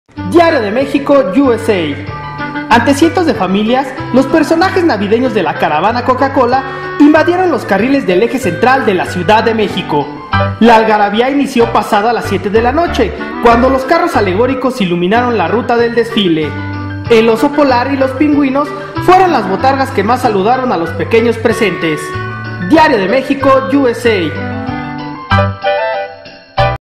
La caravana navideña deleita a México
Miles de personas asistieron este domingo al paseo de los carros alegóricos  que Coca Cola realizó en la Ciudad de México